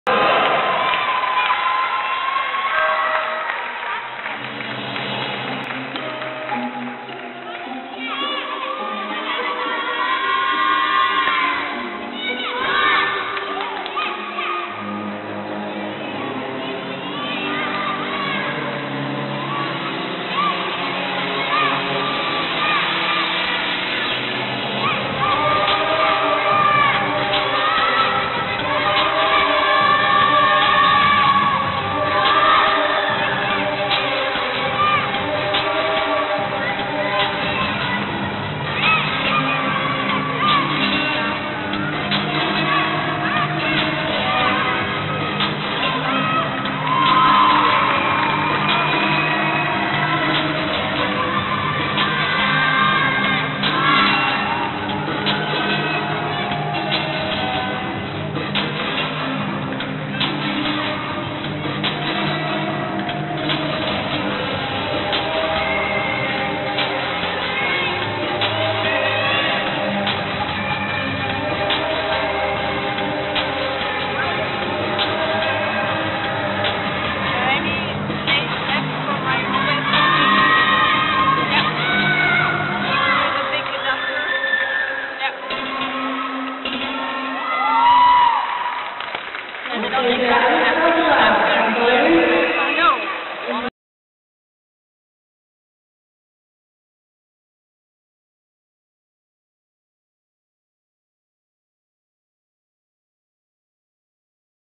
Anastasiya ALISTRATAVA (BLR) - 2018 Artistic Gymnastics Europeans, junior qualification floor
Anastasiya ALISTRATAVA (BLR) scored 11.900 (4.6) on floor in the junior qualification round of Glasgow 2018.